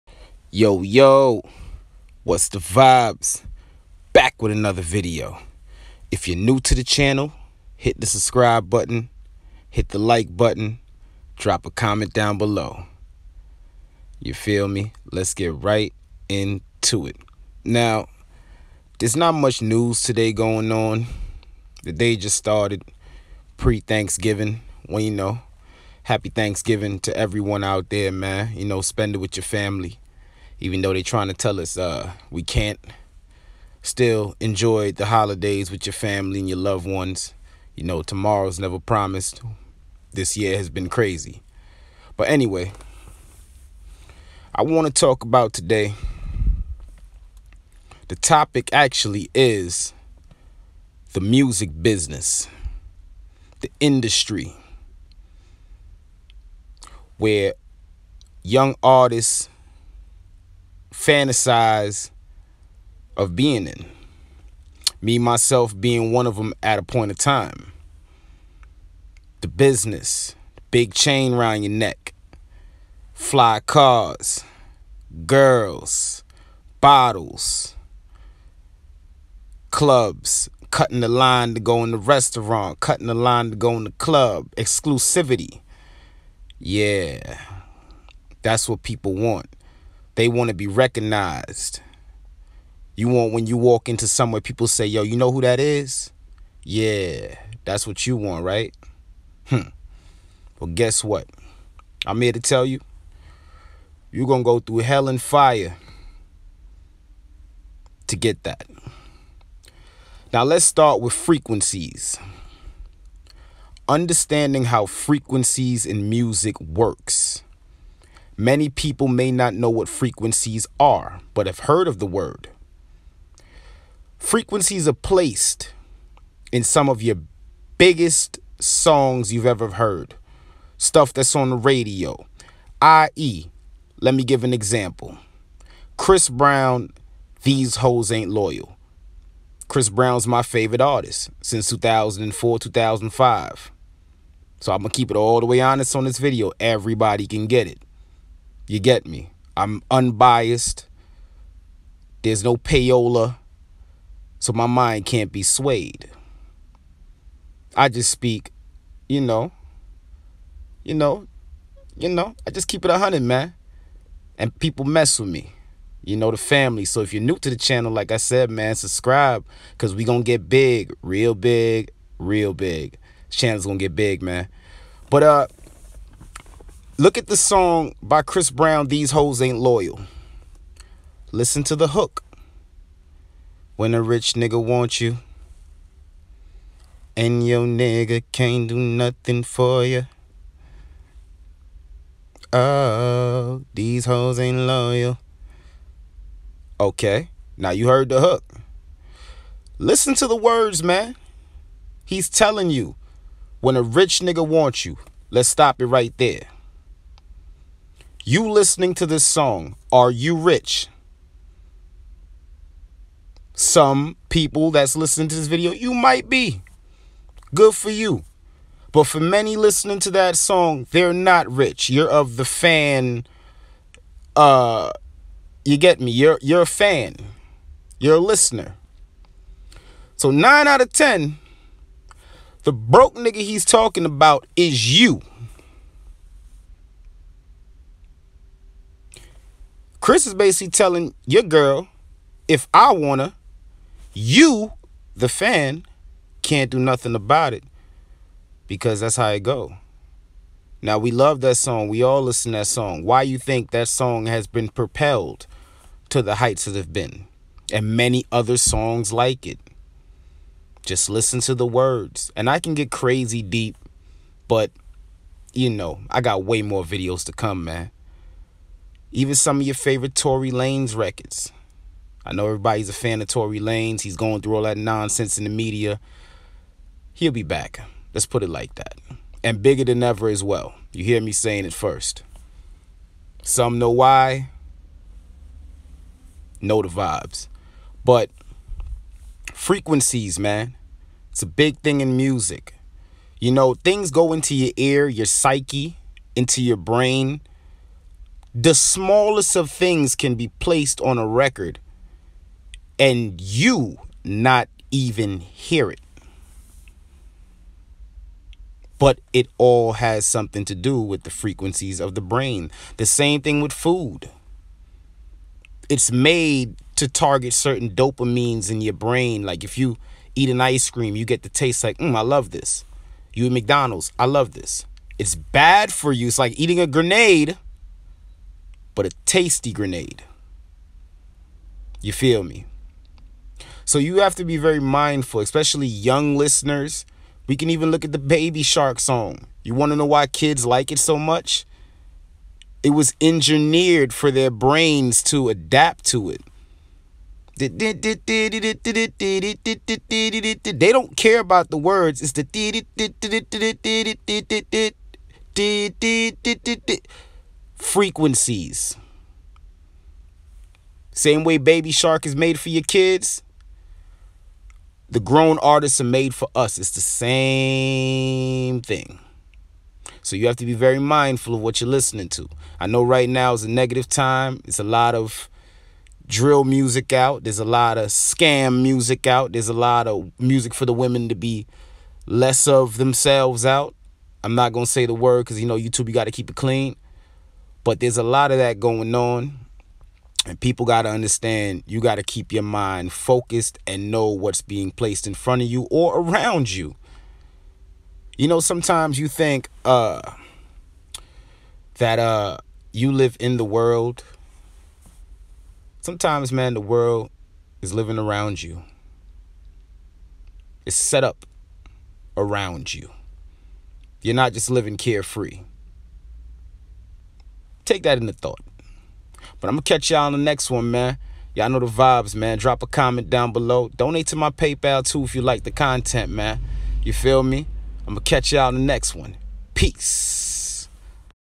MUSIC INDUSTRY 🤐
Today’s video I speak on things listeners may not know about the music business , and simply educate on how music is made . There are many things placed into music by writers and producers that an artist may not hear but the brain may pick up off of simple listening.. education is the key